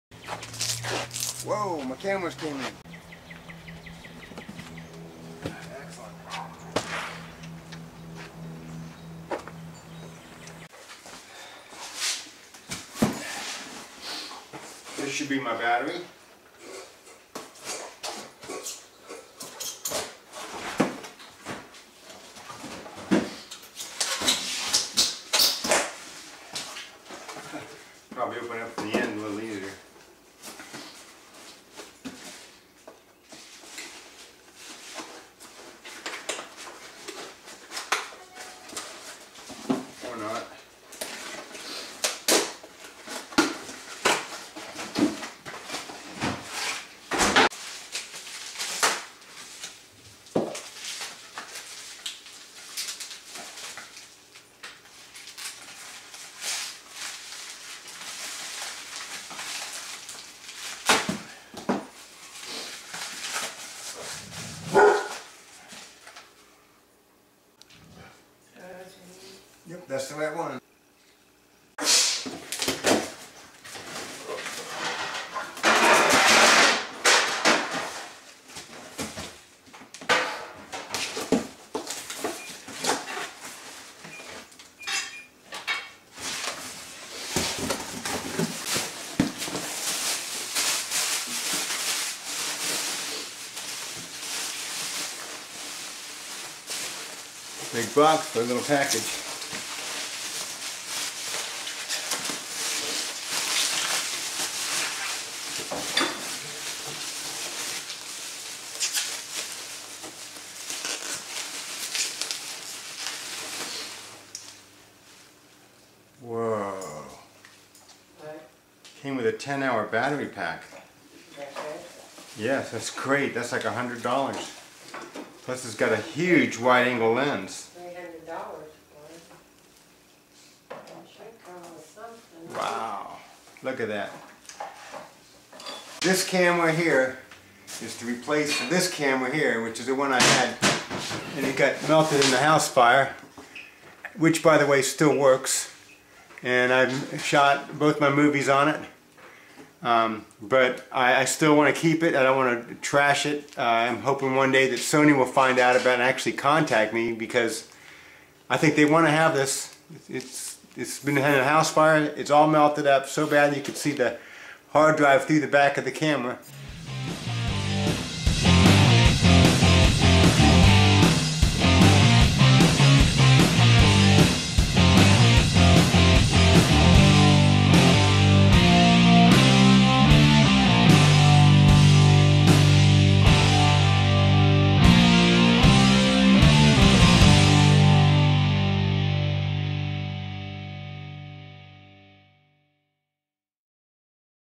Got my new Sony handycam
My old Sony that was melted in a house fire still worked, but I wanted on with an eye piece and I wanted the normal buttons for filming.  The original buttons were melted and no longer worked.  I had to open the view door to use the buttons there.